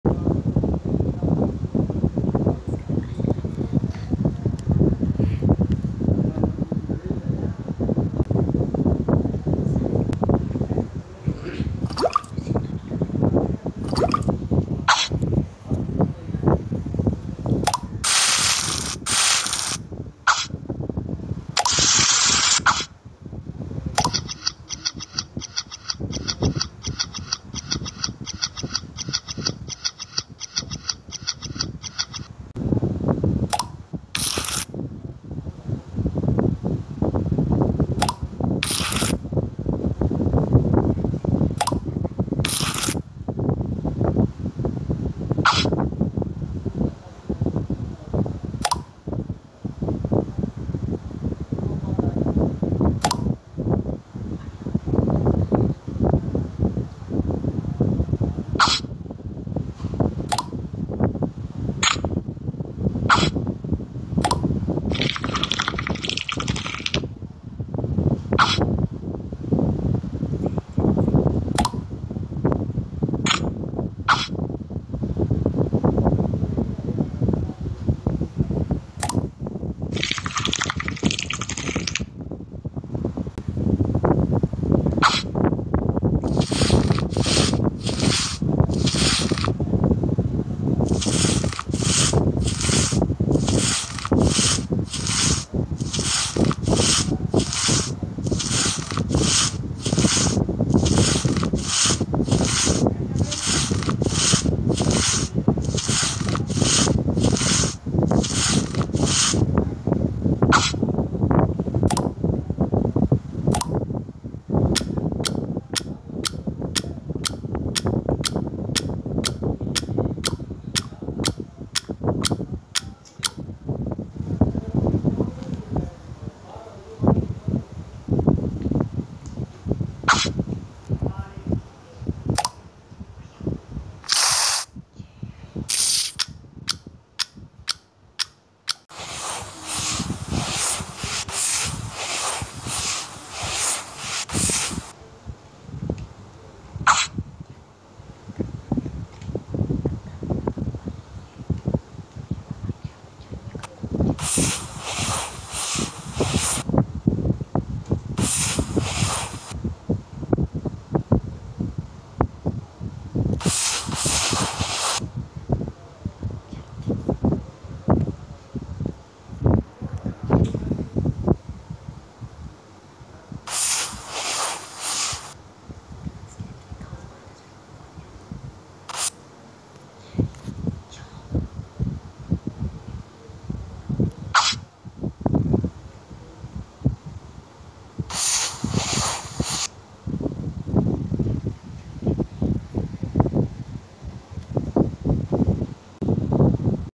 ASMR girl makeover/pimples remove & new mask apply on face animation asmr
In this video I do remove pimples on face and clean the face and stitches on face and dirty hair clean and makeover makeup Girl animation satisfying results 🥰